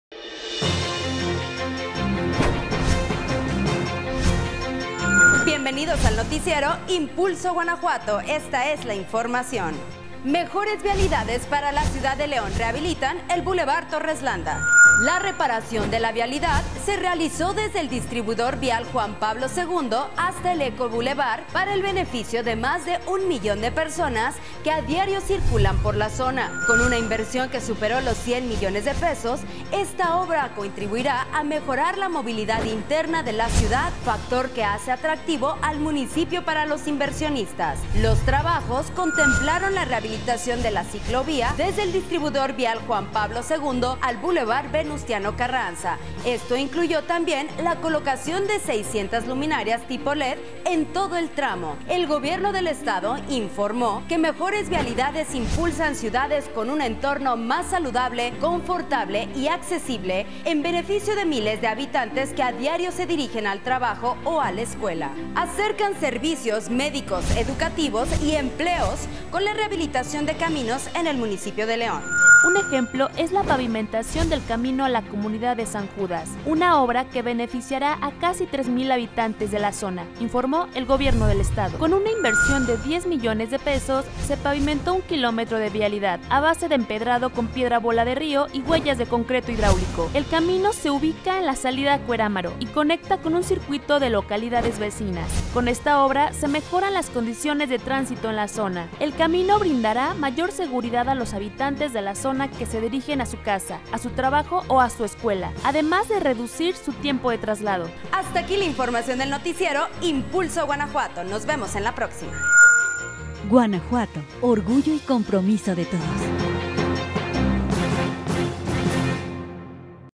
Noticiero Impulso Guanajuato | Obras de Movilidad en León
Con una inversión de 110 millones de pesos, el Gobernador, Miguel Márquez Márquez y el Acalde de León, Héctor López Santillana, entregaron la rehabilitación del Bulevar Torres Landa del tramo Distribuidor Vial Juan Pablo II-Eco Bulevar y el camino de acceso a la comunidad de San Judas.